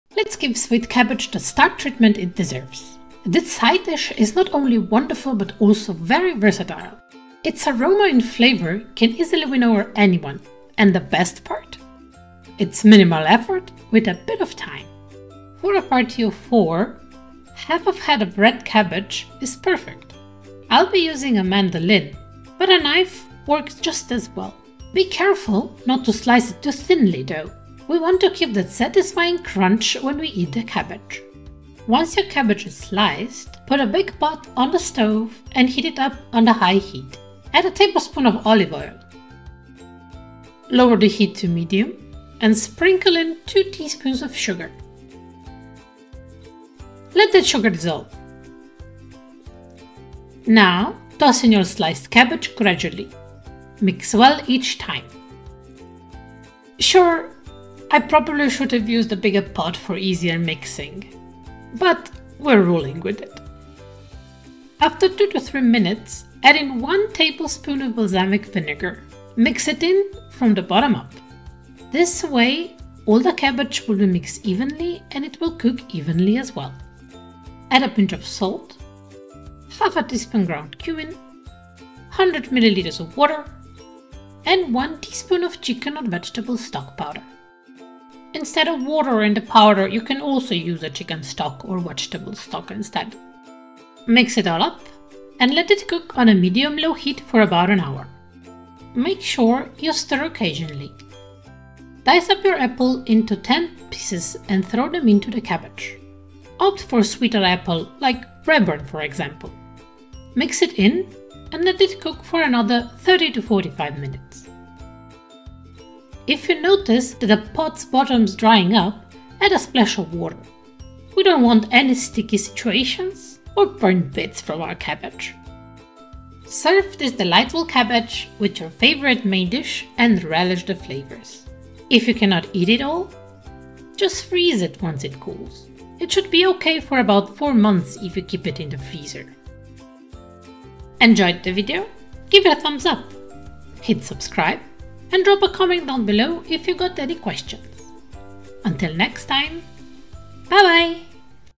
Apple-infused Braised Red Cabbage: A Deliciously Unique Side Dish!
This braised red cabbage will elevate your dish to new heights. It's very simple to make, and you can even make it in large quantities and freeze it.

📜This recipe will be good for 4 people.

🔹Ingredients:🔹
- ½ head red cabbage
- 1 tbsp olive oil

- 2 tsp sugar
- 1 tbsp balsamic vinegar

- ½ tsp ground cumin
- 1 apple

- 1 tbsp chicken / vegetable stock powder

- 100 ml / 0,4 cup water

🔹Preparation:🔹
1. Slice up your cabbage into thin strips. Use a mandolin or a knife.
2. Put a big pot onto a stove and heat it up on high heat. Add a tablespoon of olive oil, lower the heat to medium, and sprinkle in 2 teaspoons of sugar. Let that sugar dissolve.
3. Once the sugar becomes golden, add in your sliced cabbage gradually, mixing well.
4. Let it cook for 2-3 minutes. After add in 1 tablespoon of balsamic vinegar and mix it in from the bottom up.
5. Add a pinch of salt, 1/2 teaspoon ground cumin, 100ml of water, and 1 teaspoon of chicken or vegetable stock powder. Mix it all up, and let it cook on medium-low heat for about an hour. Stir occasionally.
6. Dice up your apple into 10 pieces and throw them into the cabbage. Mix it in and let it cook for another 30 to 45 minutes. If you notice the pot's bottom drying up, add a splash of water.
7. Serve.

Tip: If you can't eat all, let it cool down and freeze it for some other time.